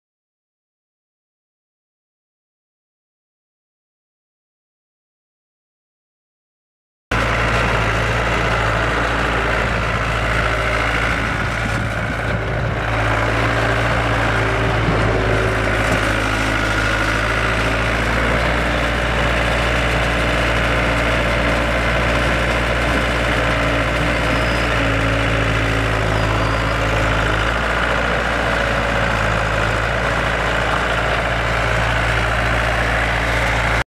2000 Case 1845C skid steer for sale | sold at auction February 13, 2014
2000 Case 1845C skid steer
1,891 hours on meter
3.92L four cylinder diesel engine
60 HP
Hydrostatic transmission
Joystick bucket controls
72" bucket
Manual quick coupler
Auxiliary hydraulics
Denman 12-16.5 tires
Serial JAP0315890

It was located in St. Joseph, MO, and we sold it to the highest bidder for $14,300.